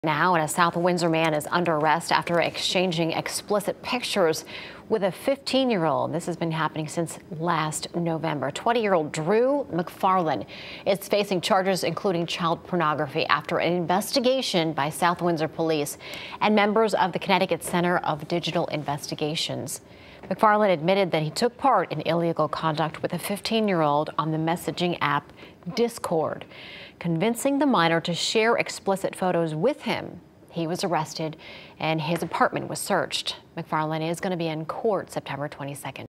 South Windsor man arrested, charged after soliciting explicit photos from Arizona teen
South Windsor police said Drew McFarland met a 15-year-old from Arizona on the messaging app Discord and convinced the teen to send explicit photos.